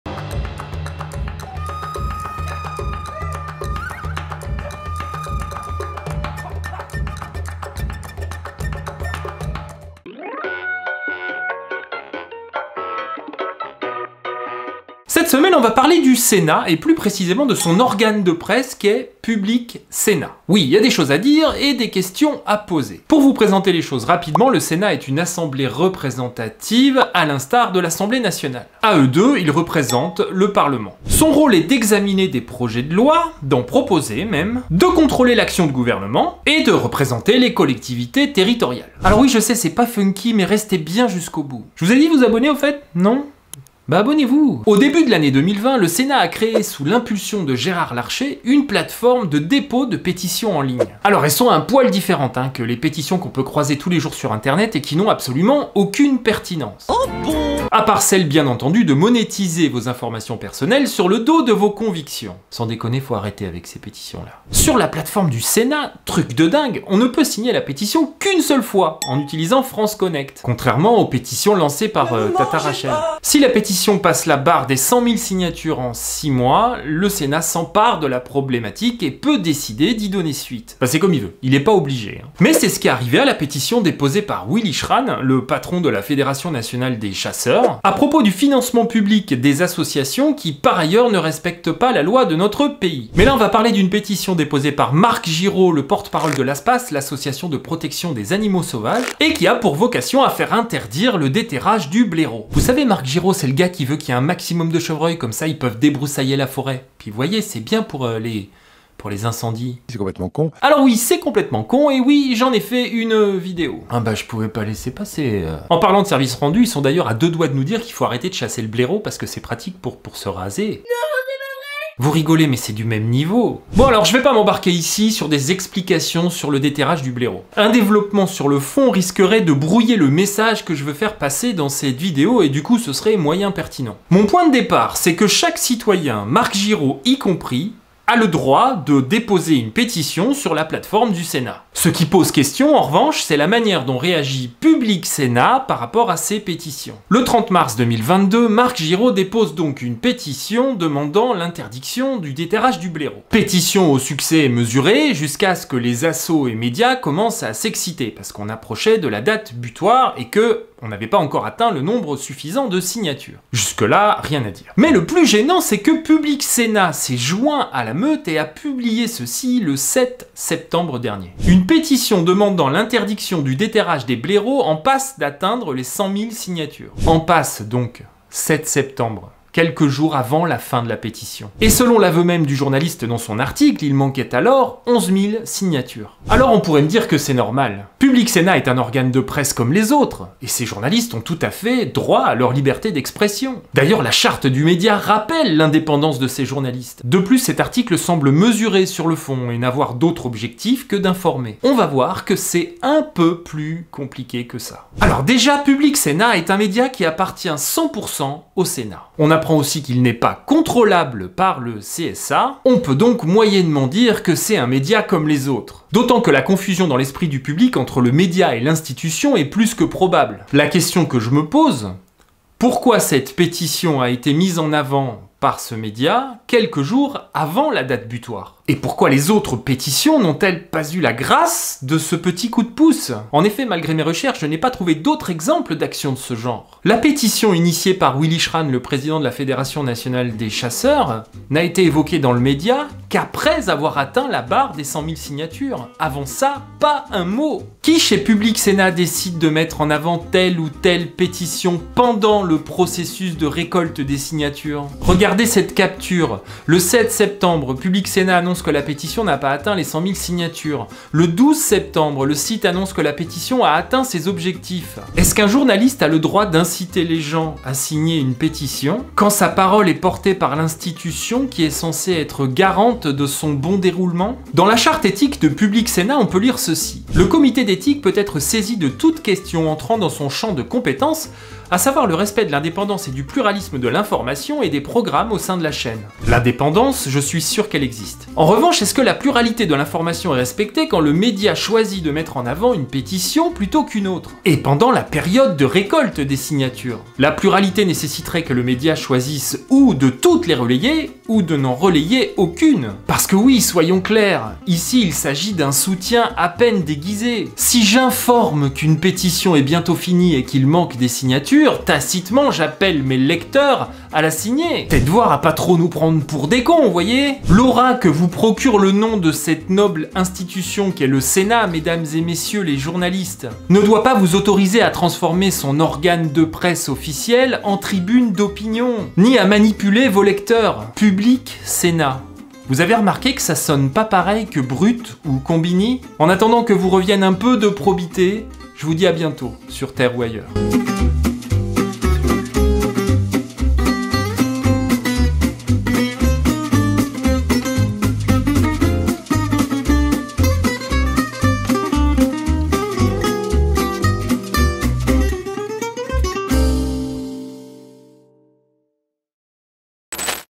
Public Sénat anti-chasse ?
@publicsenat anti-chasse ?
Il me semble que quand on est chargé de rendre compte des activités du sénat, il devrait exister un devoir de réserve, notamment au sujet des pétitions dont l'institution se porte garante.
Ma vidéo sur les grandes idées de Marc Giraud, porte parole de l'@ASPASNature

Regardez cette vidéo : "Tondelier : une écolo vegan anti nucléaire ne devrait pas dire ça "